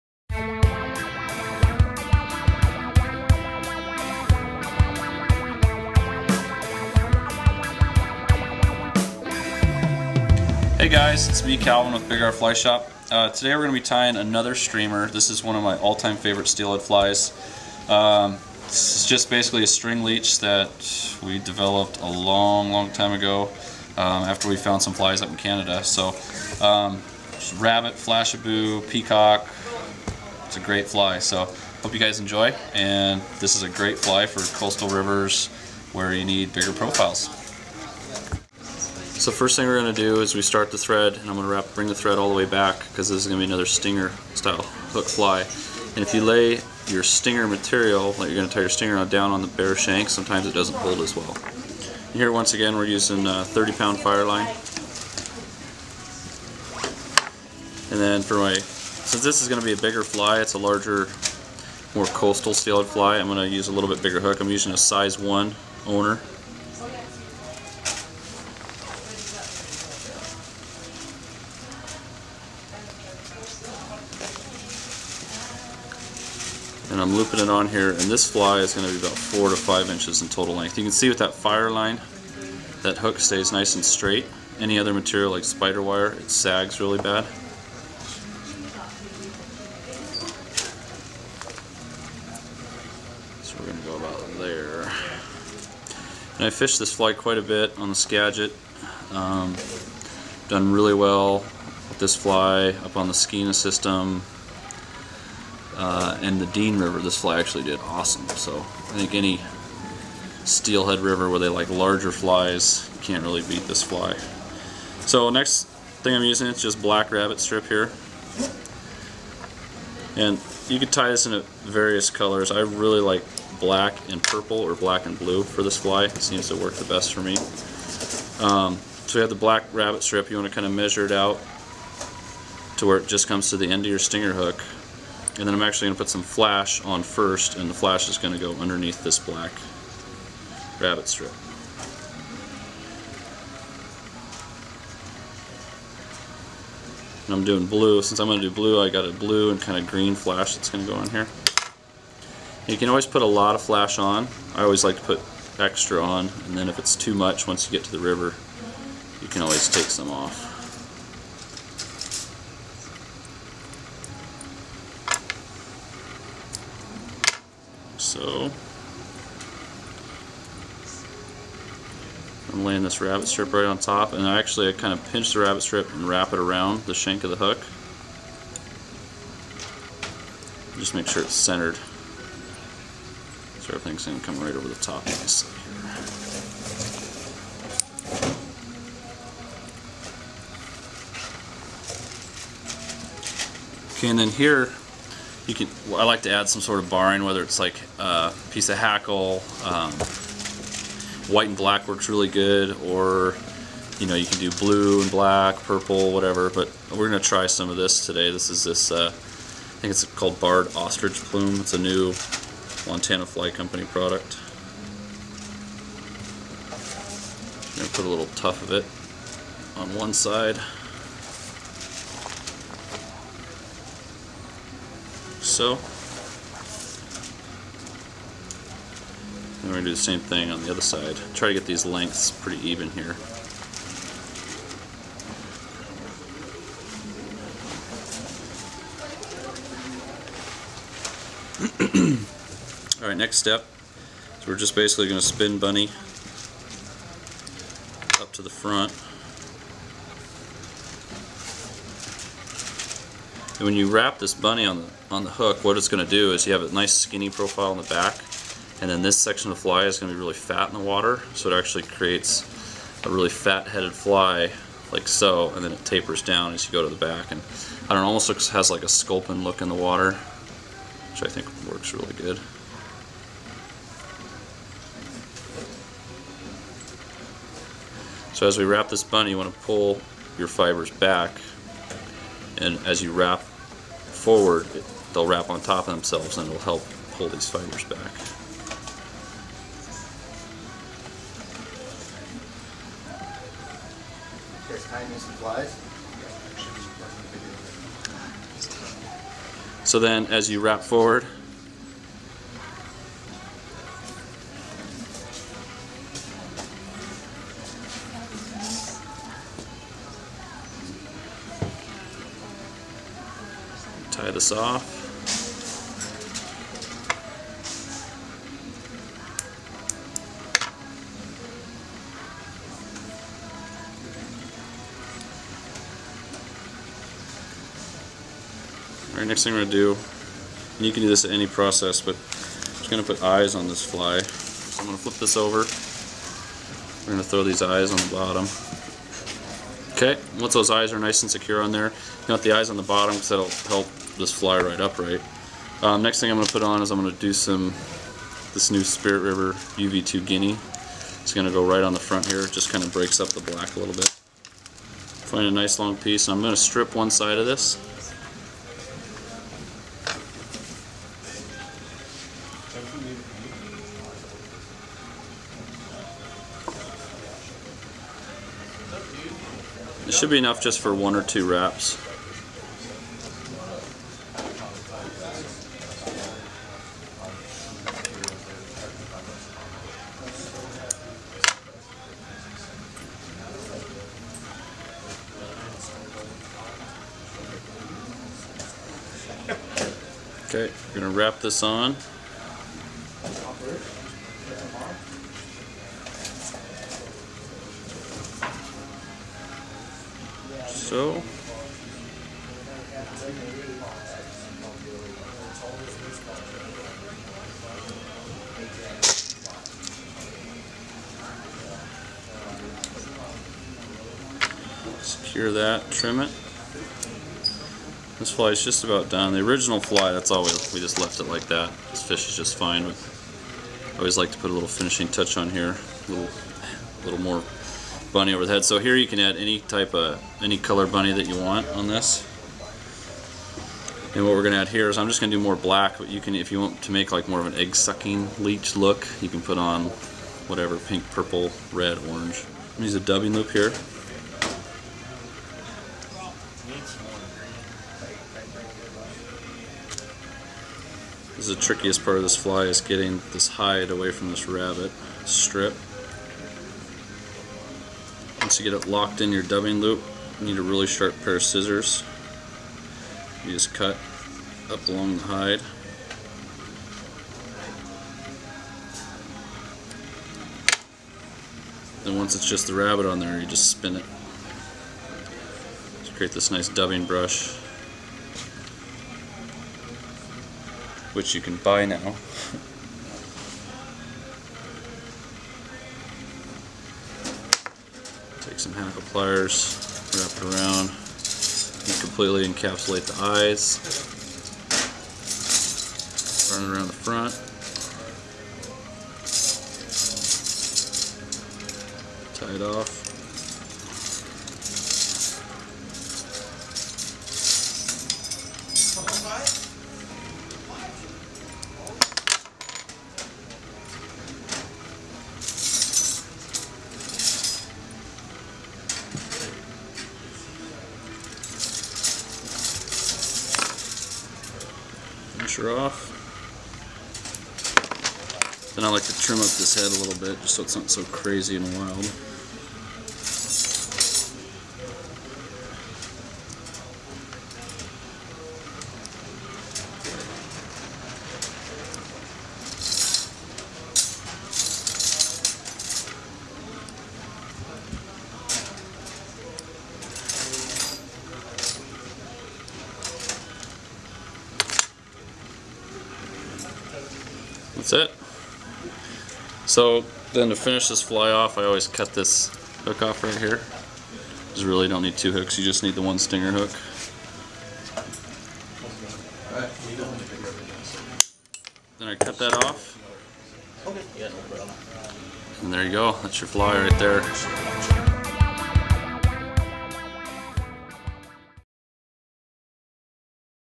Fullers Fortune
This fly was developed to fish coastal river for steelhead or rivers where fish like a large profile fly. It is an intruder style fly with the stinger hook, but it is not considered an actual intruder.

We started tying this fly back in 2001 and it has worked great on the Skagit, Dean, Skeena, and many others.

Nicknamed "Fuller's Fortune" because the first fish I caught on this fly was a 38" steelhead out of the Nooksack River in Washington. Right after I landed the fish I found a road sign on the beach that said "Fortune Lane" That is now the name of the run on the Nooksack River.